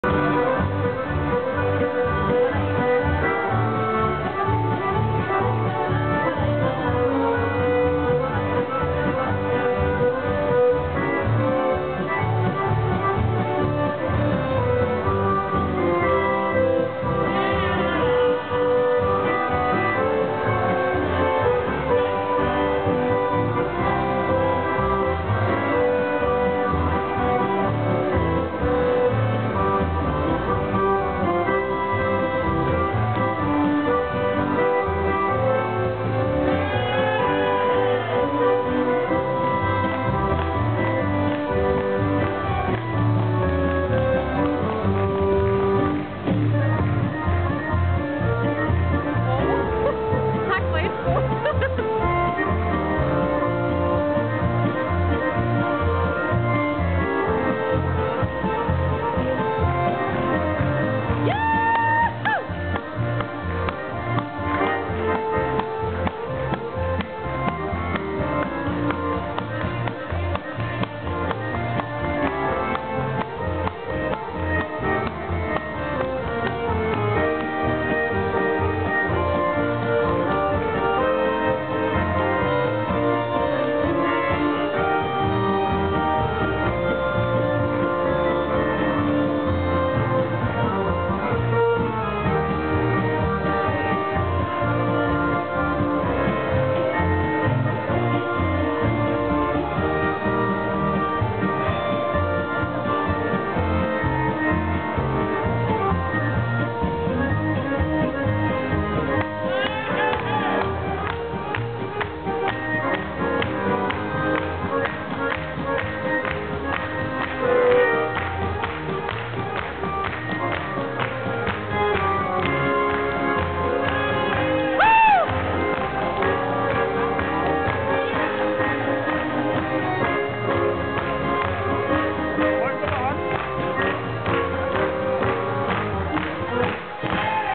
Lindqvisterna i Bindslev
Folke Lindqvist og Anders Nilsson spiller med sitt komp på Bindslev Harmonikafestival